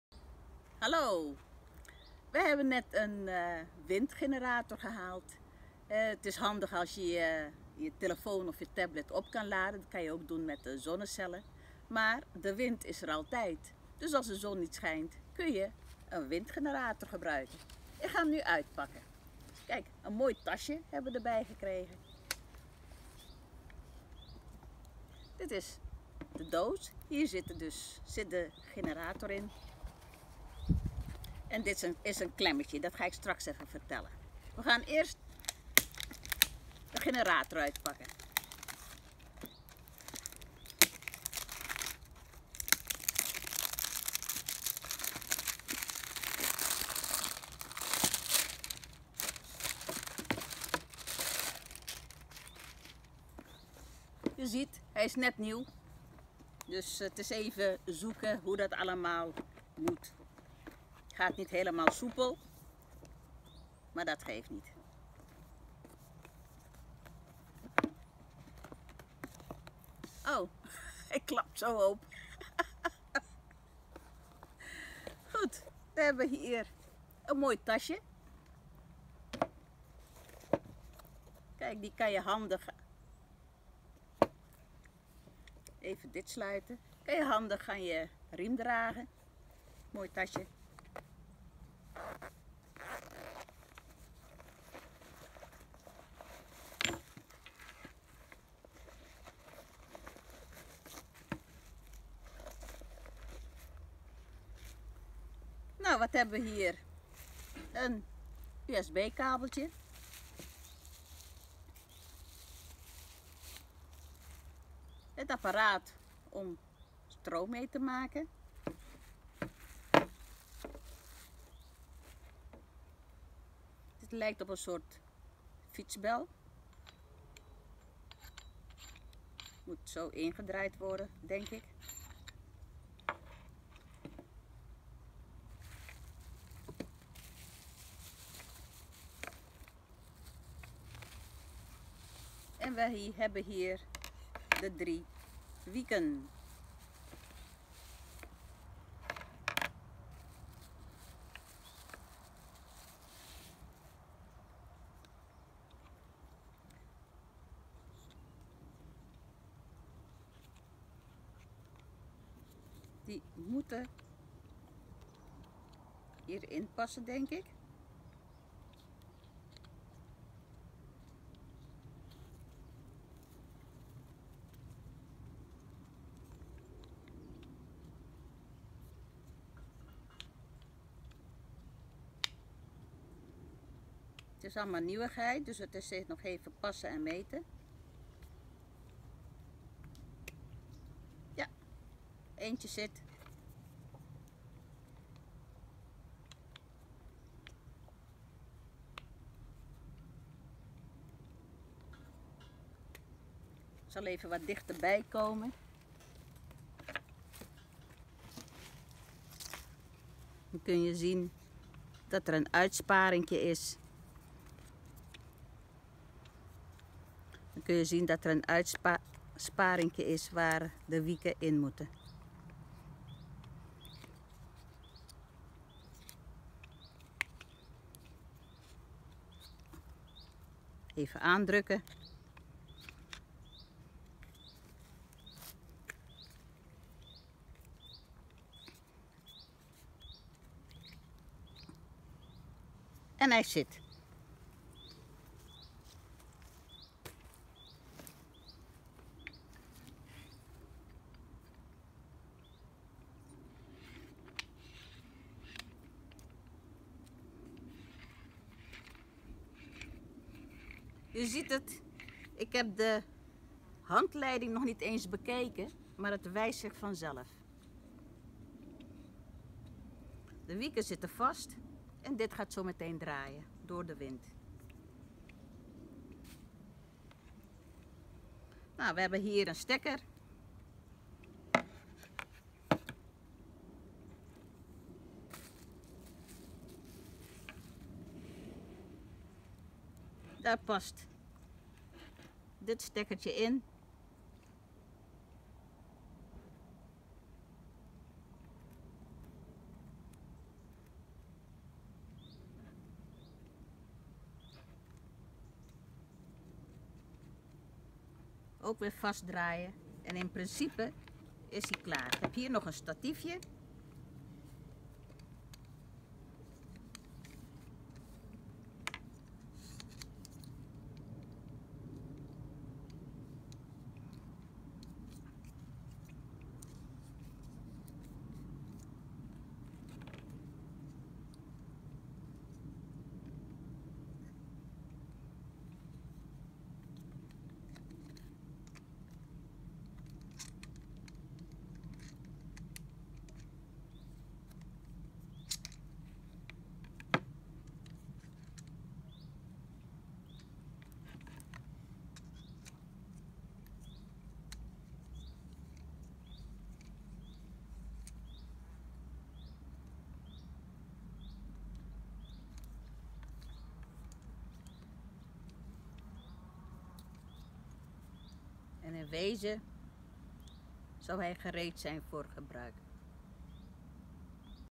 Unboxing uitpakken windmolen / off grid unboxing our new wind generator
Sieglien pakt windmolen uit. Geweldige hulp bij losgekoppeld leven. Altijd stroom, als het maar waait. Sieglien unboxing a wind generator. Perfect for off grid living.
Always electrical power on our campsite with this wind generator.